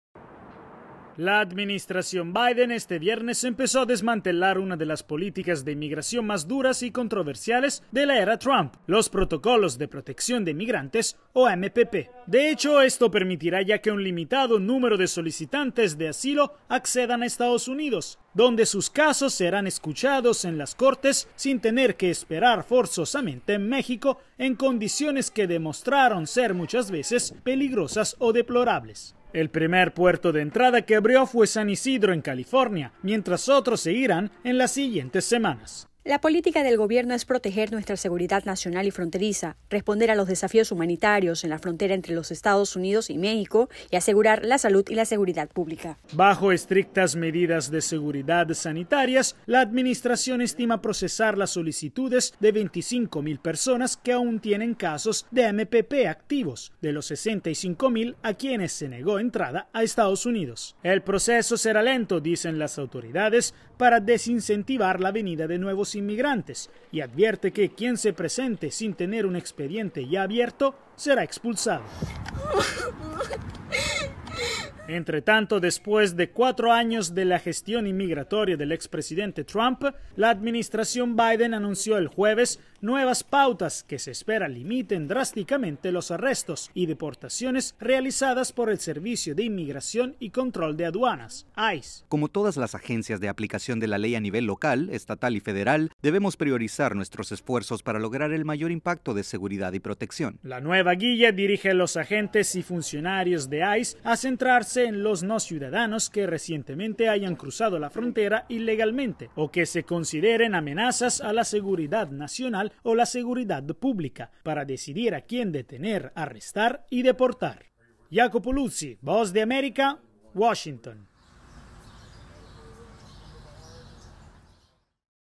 EE.UU. vuelve a aceptar a solicitantes de asilo en su territorio
El gobierno del presidente Biden está revolucionando las políticas inmigratorias de Estados Unidos. Además de la ambiciosa reforma propuesta en el Congreso, muchas otras cosas están cambiando; por ejemplo, la manera de procesar las solicitudes de asilo y cómo el Servicio de Inmigración y Control de Aduanas trabajará en las detenciones y deportaciones de los indocumentados. Nos cuenta Iacopo Luzi.

¡Visítenos!